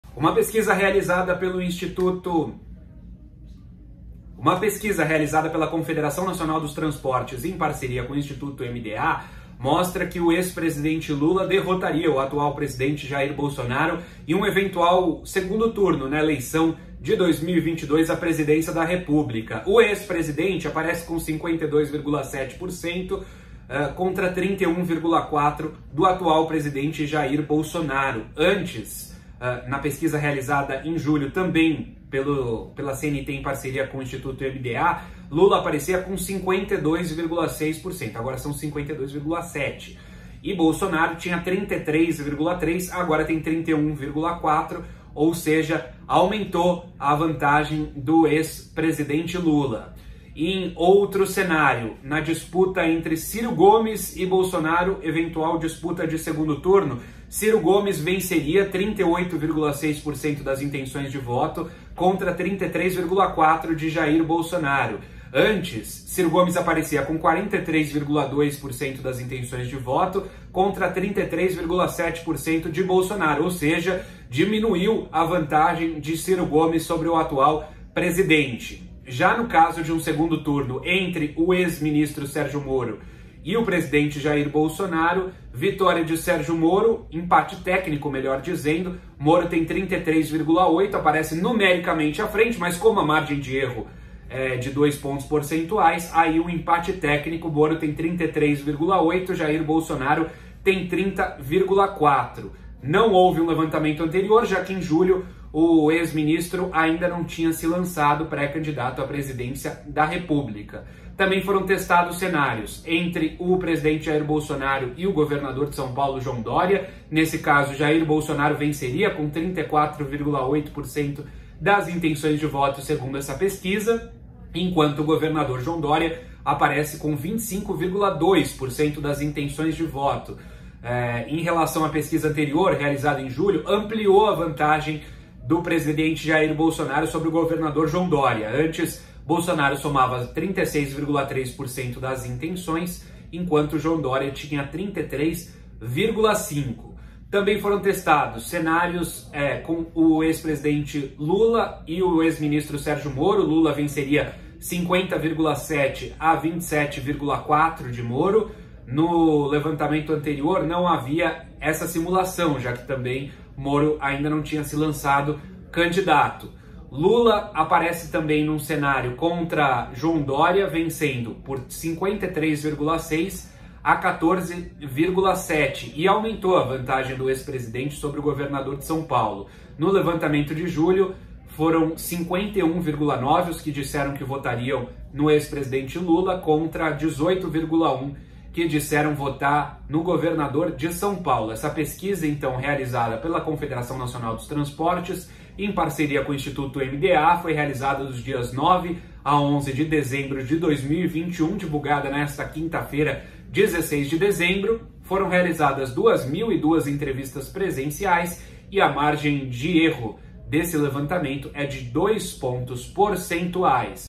Lula derrotaria Bolsonaro no 2º turno nas eleições de 2022, diz CNT
Ex-presidente aparece na frente em cenários contra Jair Bolsonaro, Sergio Moro e João Doria. Bolsonaro derrotaria Doria, enquanto Sérgio Moro e Ciro Gomes venceriam o atual presidente.